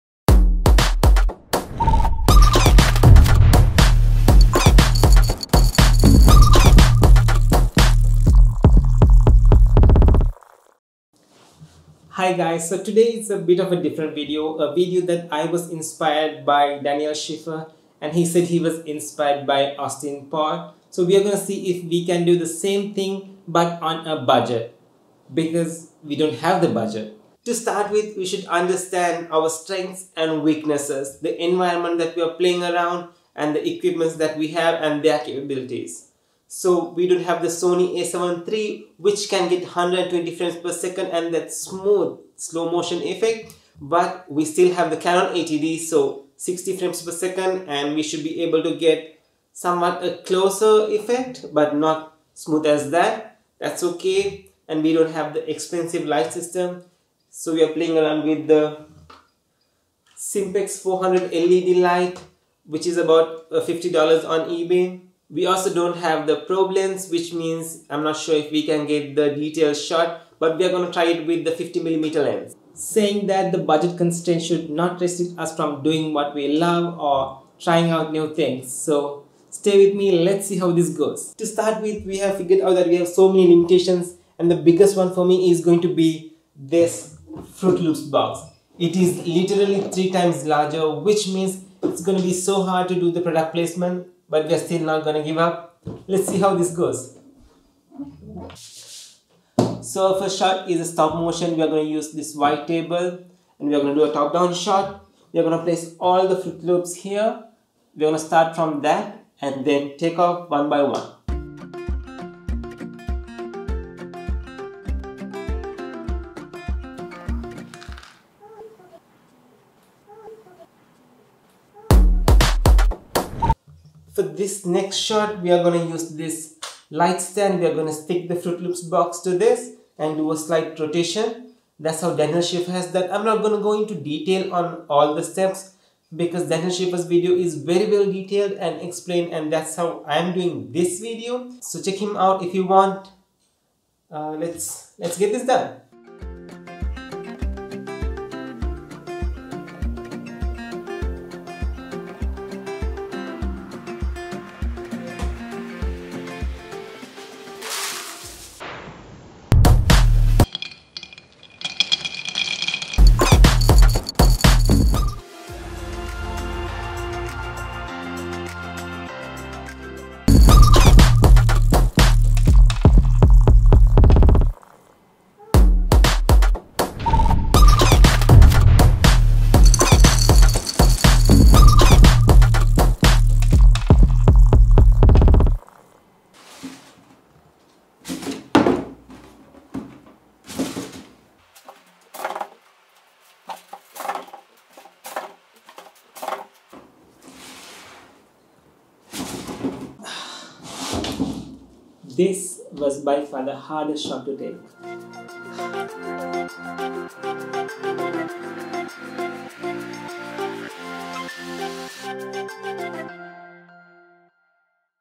Daniel Schiffer Commercial on a BUDGET setup
I recreated this to see if we can still to this cereal commercial Daniel Schiffer did, but on a very minimal budget setup. What do you guys think?